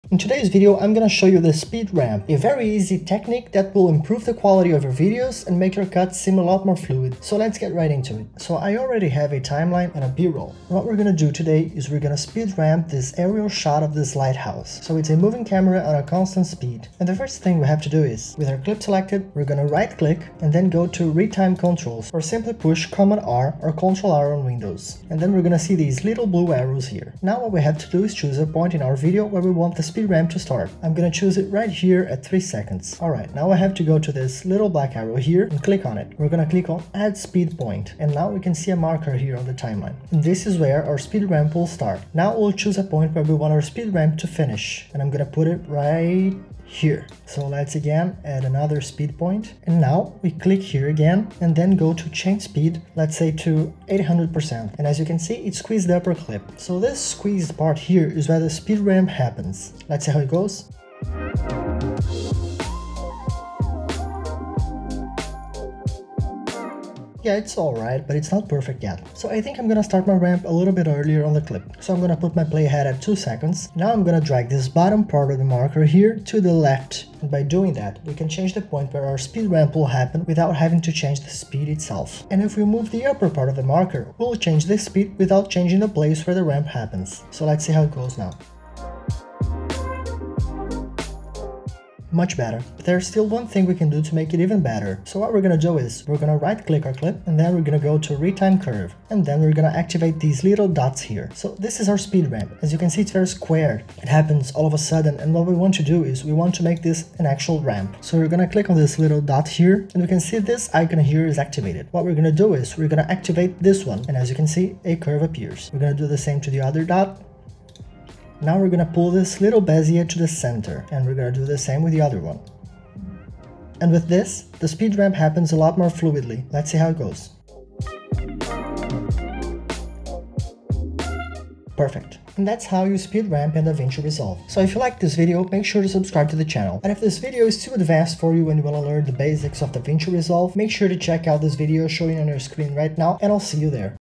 The BEST way to SPEED RAMP in DaVinci Resolve | Tutorial
In today's video I'm gonna show you how to speed ramp in DaVinci Resolve, which is a very simple yet powerful technique to make your videos look a lot better.

With this basic technique you'll learn how to change the speed of your clip inside the edit page, and I'm gonna show you all the tools to make your speed ramp look very smooth by messing around with the speed points.